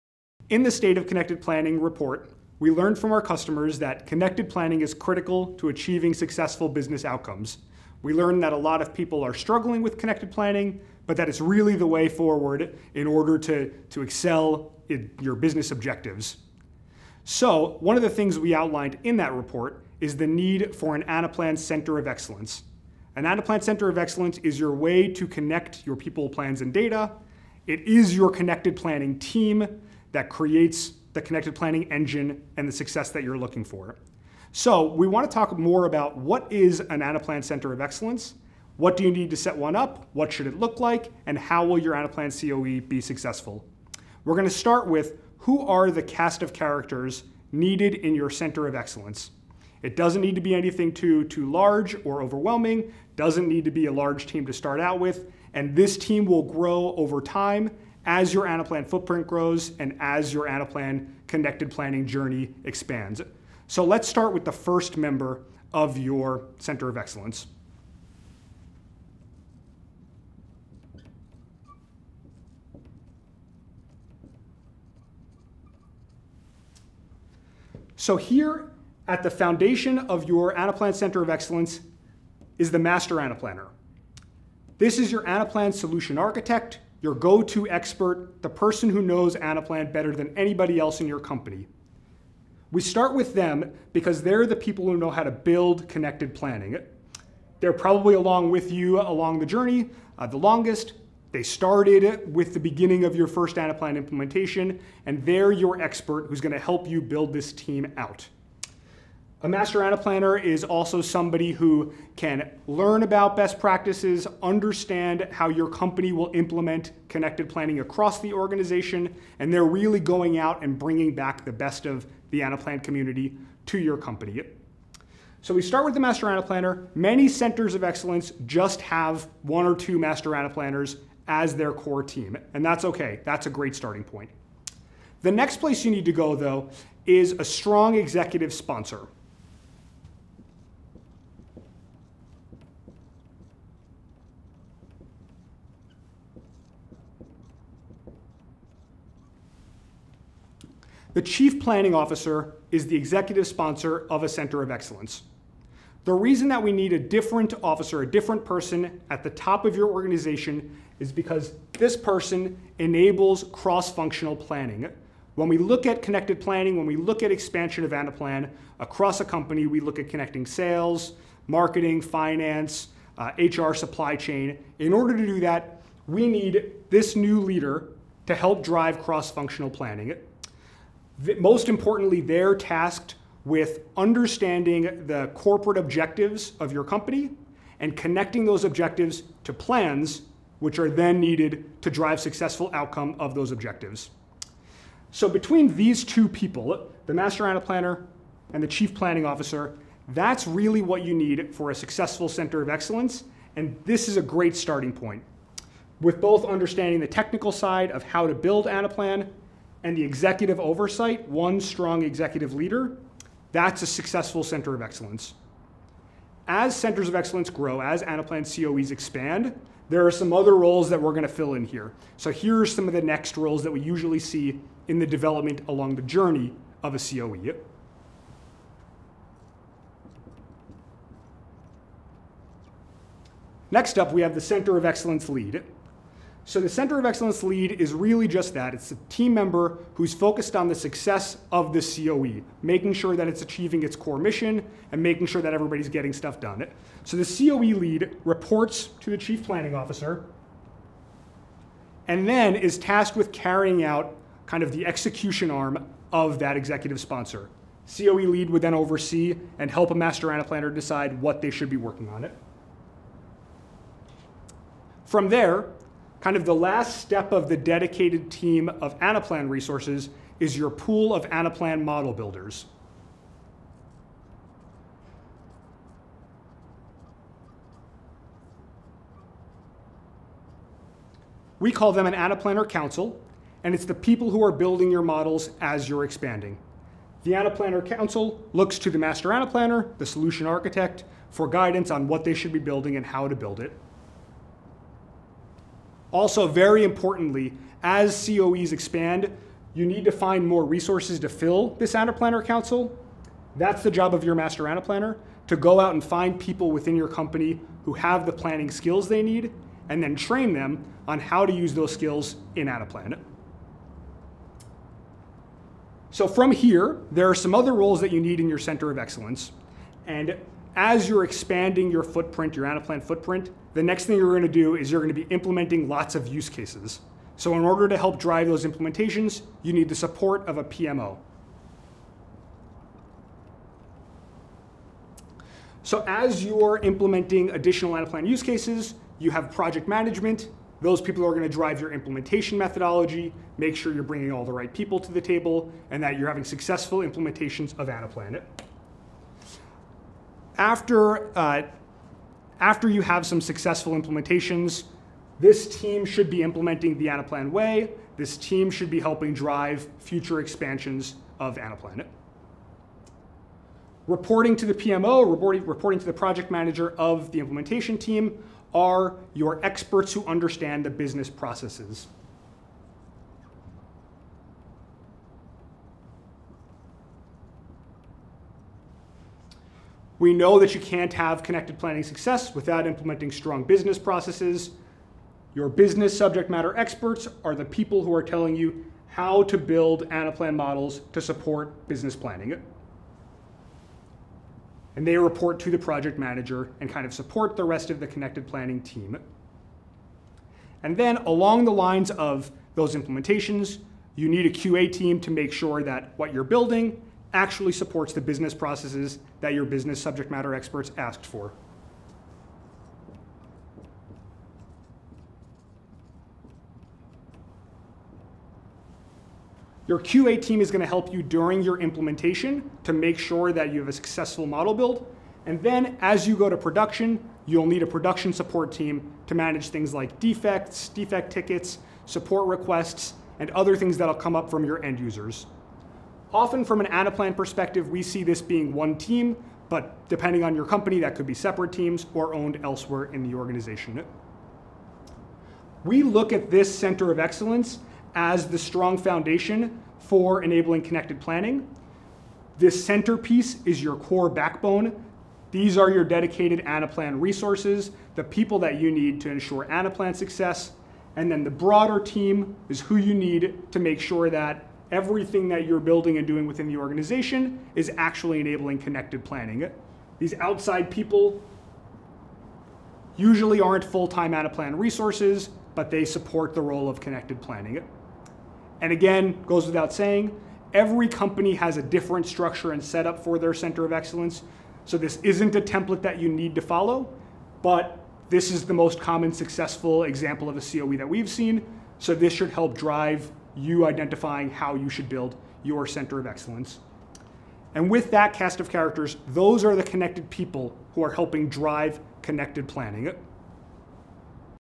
The Center of Excellence Team
Learn how to structure the cast of characters needed on your Anaplan Center of Excellence to enable and support your Connected Planning journey.